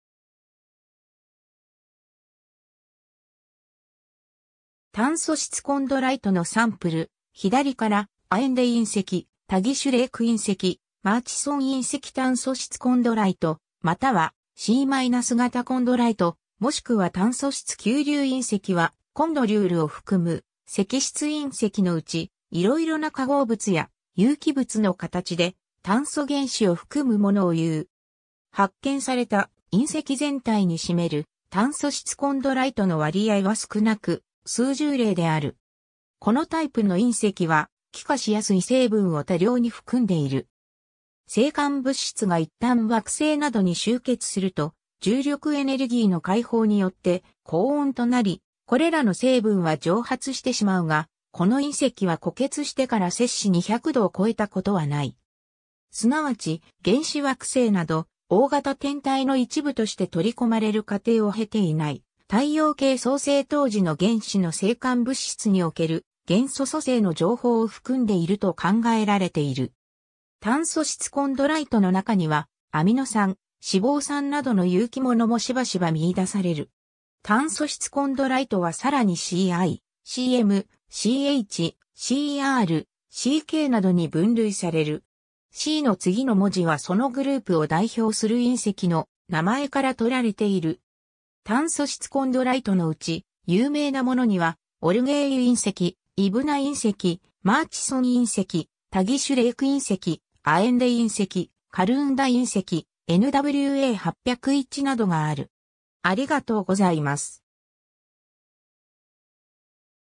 炭素質コンドライト
炭素質コンドライトのサンプル、左から、アエンデ隕石、タギシュ・レイク隕石、マーチソン隕石 炭素質コンドライト（たんそしつコンドライト、Carbonaceous Chondrite）、またはC-型コンドライト、もしくは炭素質球粒隕石はコンドリュールを含む石質隕石のうち、いろいろな化合物や有機物の形で炭素原子を含むものをいう。
発見された隕石全体に占める炭素質コンドライトの割合は少なく、数十例である。
 このタイプの隕石は、気化しやすい成分を多量に含んでいる。
星間物質がいったん惑星などに集結すると重力エネルギーの開放によって高温となり、これらの成分は蒸発してしまうが、この隕石は固結してから200℃を超えたことはない。
すなわち、原始惑星など大型天体の一部として取り込まれる過程を経ていない太陽系創生当時の原始の星間物質における、元素組成の情報を含んでいると考えられている。
炭素質コンドライトのなかには、アミノ酸、脂肪酸などの有機物もしばしば見出される。
 炭素質コンドライトはさらにCI、CM、CH、CR、CKなどに分類される。
Cのつぎの文字はそのグループを代表する隕石の名前から取られている（例としてはCIのIはイブナ隕石にちなむ）。
炭素質コンドライトのうち、有名なものにはオルゲイユ隕石、イブナ隕石、マーチソン隕石、タギシュ・レイク隕石、アエンデ隕石、カルーンダ隕石、NWA 801などがある。